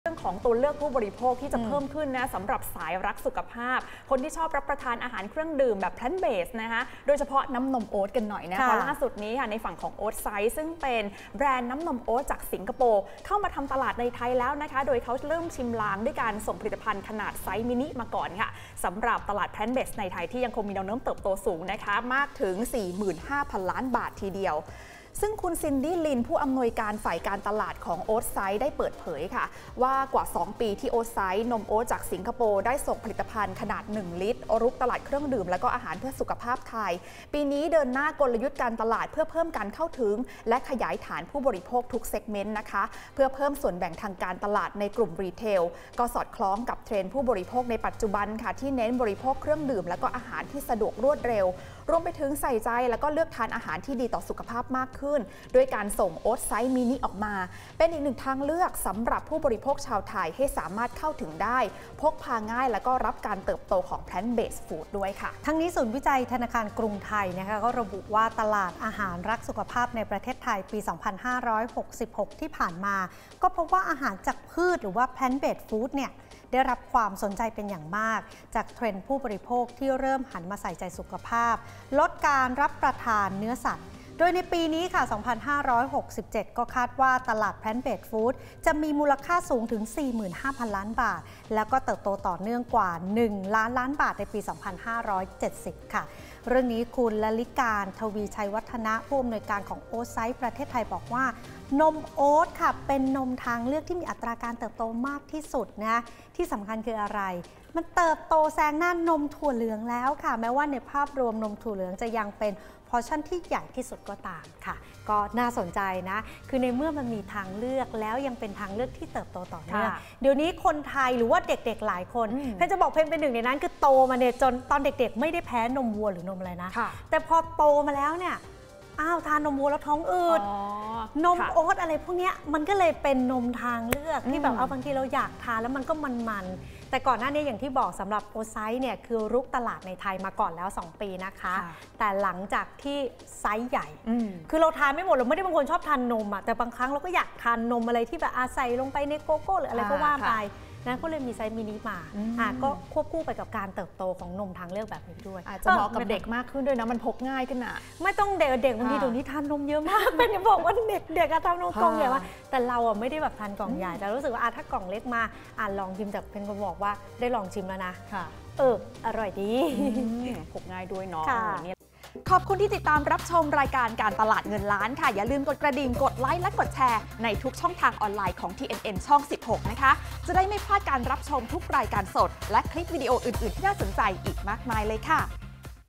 โตไม่หยุด! "OATSIDE" ส่งไซส์มินิชิงส่วนแบ่งค้าปลีก | การตลาดเงินล้าน 17 มิ.ย. 67
ตลาดอาหารรักสุขภาพในประเทศไทยเติบโตต่อเนื่อง อาหารจากพืช (Plant-based Food) ได้รับความสนใจเป็นอย่างมาก จากเทรนด์ผู้บริโภคที่เริ่มหันมาใส่ใจสุขภาพ ล่าสุด OATSIDE แบรนด์นมโอ๊ตจากสิงคโปร์ส่งกล่องไซส์มินิ ลุยตลาดค้าปลีก รับการเติบโต Plant based-Food ที่มีมูลค่า 4 หมื่น 5 พันล้านบาท